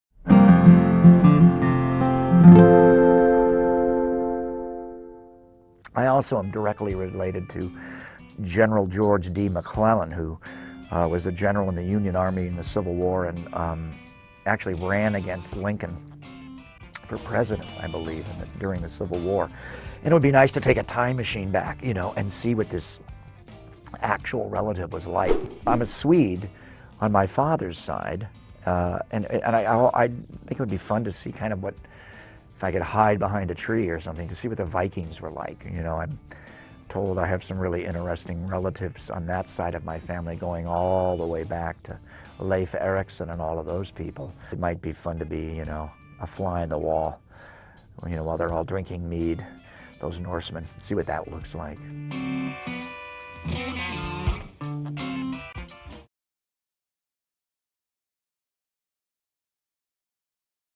Watch Robert Englund Talk About Going Back in Time
If Robert Englund could go back in time...